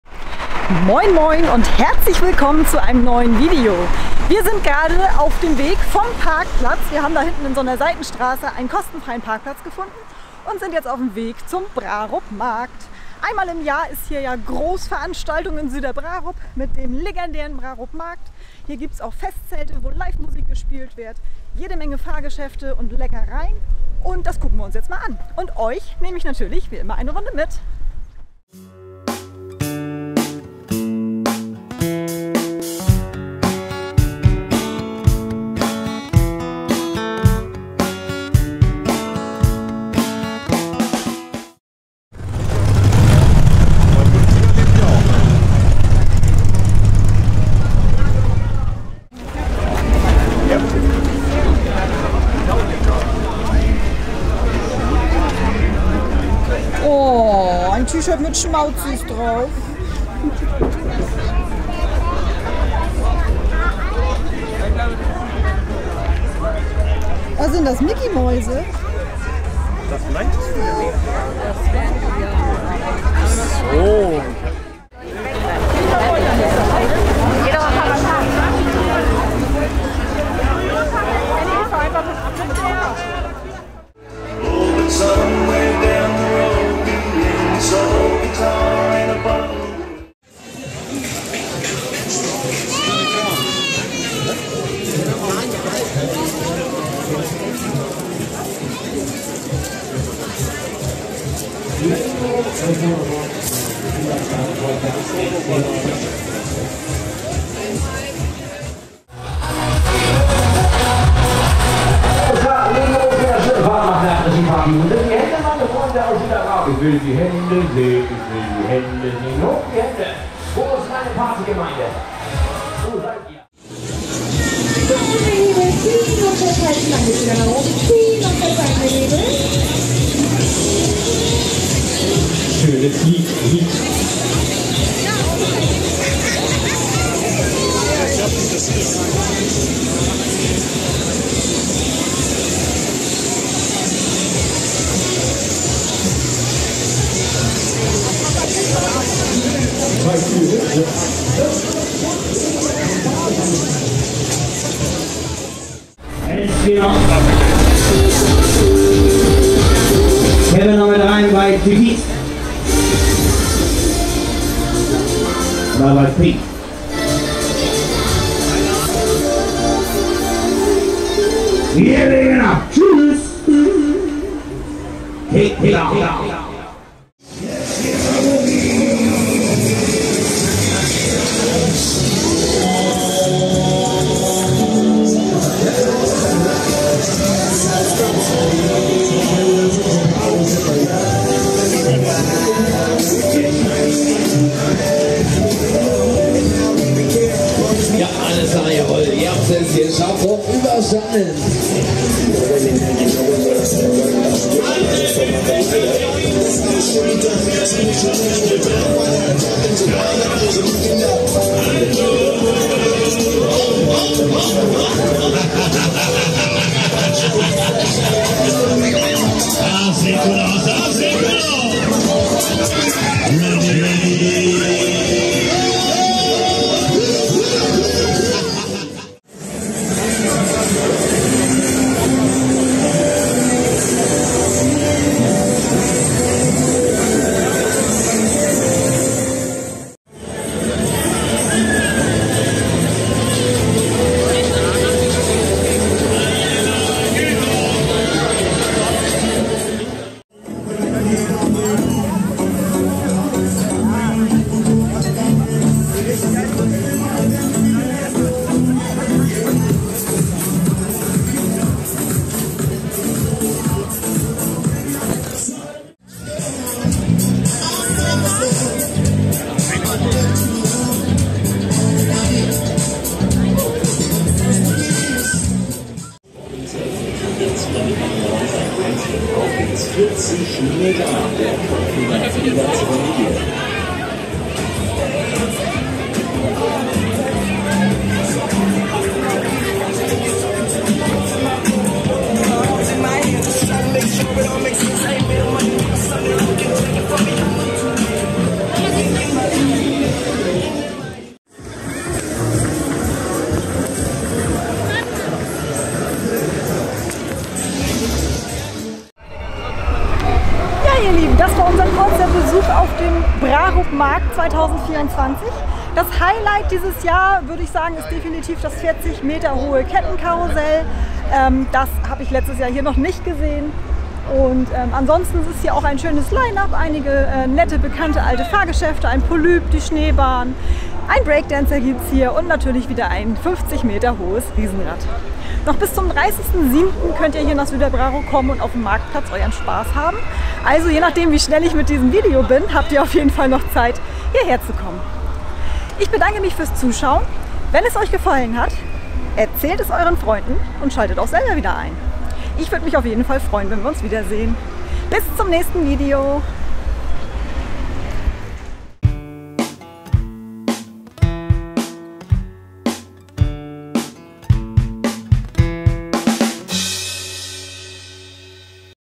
Jahrmarkt in Süderbrarup: Brarup Markt 2024
Vom 26.07. bis zum 30.07 war wieder Jahrmarkt in Süderbrarup.

Natürlich habe ich es mir nicht nehmen lassen und habe dort auch eine kleine Runde gedreht. dieses Jahr ist der 40 m hohe Kettenflieger das Highlight.

Es gab wieder eine Menge Live-Musik, viele Fahrgeschäfte und ganz viel zu essen.

ich wünsche euch viel Spaß mit diesem Video!